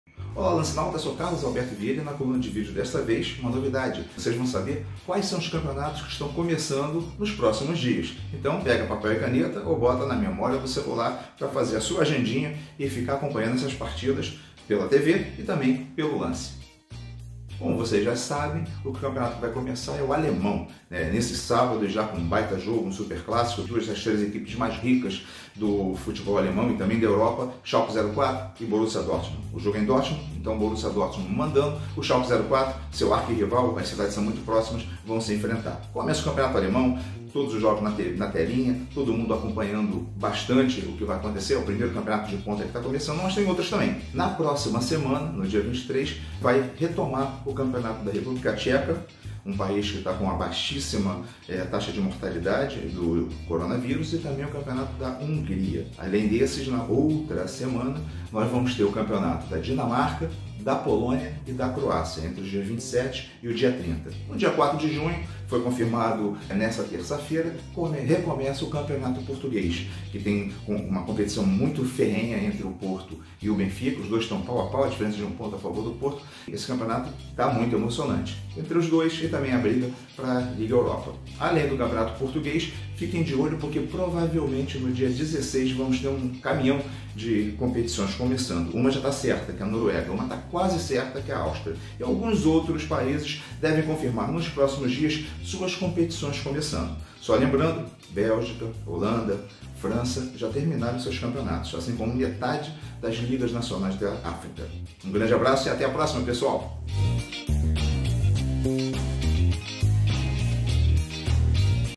Editor do L! projeta duelos no retorno das ligas europeias
O editor do L!, Carlos Alberto Vieira, analisa o retorno de parte das ligas europeias e projeta duelos na Alemanha e Portugal